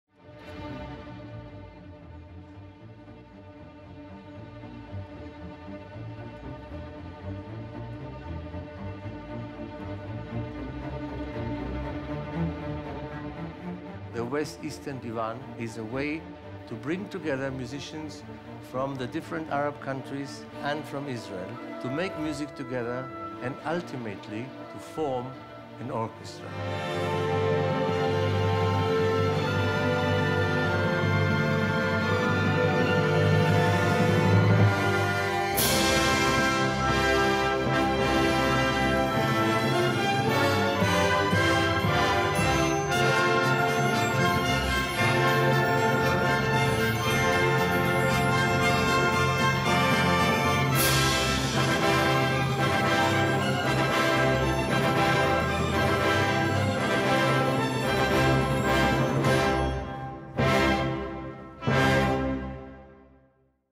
Trailer - Waldbühnenkonzert 2013
25 AUGUST, 2013: The culmination of the West-Eastern Divan Orchestra's Summer 2013 Tour. Music by Verdi, Wagner, and Berlioz. Tickets

Der Höhepunkt der Sommertournee 2013 des West-Eastern Divan Orchestra. Werke von Verdi, Wagner und Berlioz